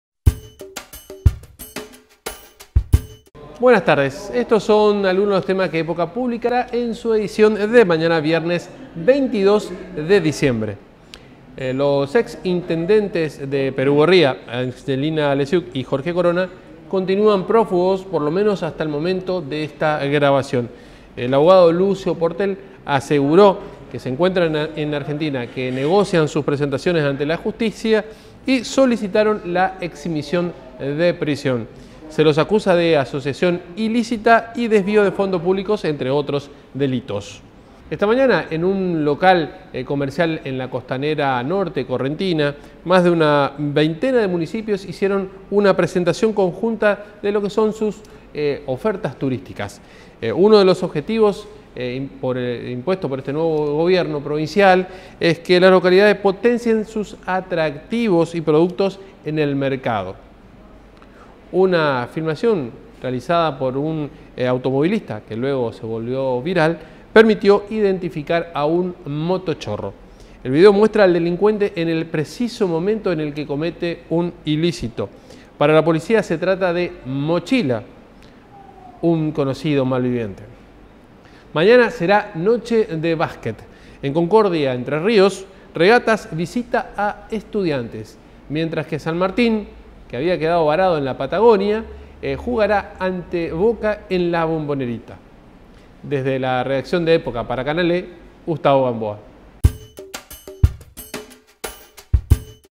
Anticipo de noticias - 22 - 12 - 2017
¿Querés enterarte antes sobre los temas que desarrollará Diario época en su edición de mañana? Los periodistas del matutino te cuentan en este adelanto.